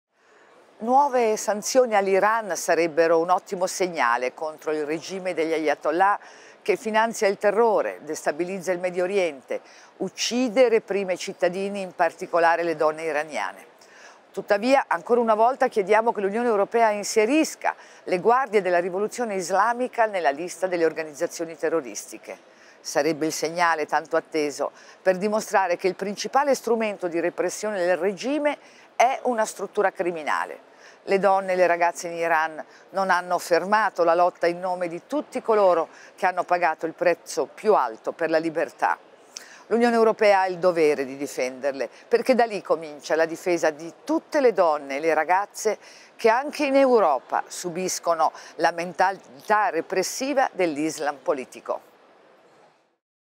Nuove sanzioni all’Iran sarebbero un ottimo segnale contro al Regime degli Ayatollah
Nuove sanzioni all’Iran sarebbero un ottimo segnale contro al Regime degli Ayatollah che finanzia
il terrore, destabilizza il Medio Oriente, uccide e reprime le donne iraniane.
Tuttavia, ancora una volta chiediamo che L’Unione Europea inserisca le Guardie della rivoluzione
islamica nella lista delle organizzazioni terroristiche. Sarebbe il segnale, tanto atteso, per
dimostrare che il principale strumento di repressione del Regime è una struttura criminale.
Le donne e le ragazze in Iran non hanno fermato la lotta in nome di tutti coloro che hanno pagato
il prezzo più alto per la libertà.
L’Unione Europea ha il dovere di difenderle, perché da lì comincia la difesa di tutte le donne e le
ragazze che anche in Europa subiscono la mentalità repressiva dell’Islam politico.